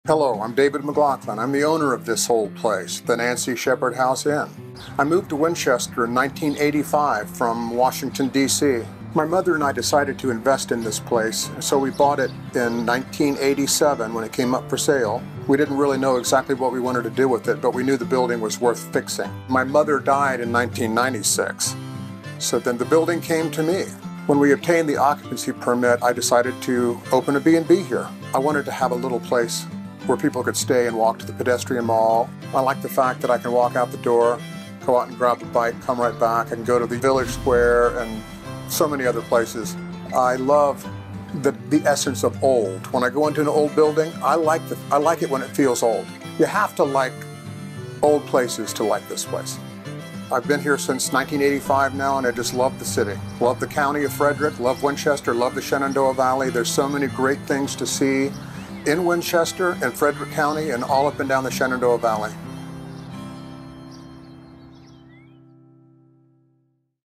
Nancy Shepherd House Inn in Winchester, VA
WE'D LOVE TO WELCOME YOU INSIDE the Nancy Shepherd House located just steps from Old Town Winchester, VA! This is a truly unique inn experience located in one of our city's oldest homes.

We'll take you inside for a quick tour with this video and introduce you to owner David McLaughlin, who also happens to be a wedding officiant AND and world-class bluegrass musician. He was inducted into the Bluegrass Music Hall of Fame & Museum in 2020 with the rest of his band, the Johnson Mountain Boys.

Big thanks to local videographer Cedarmeade Studios for another great look at our local businesses and their owners.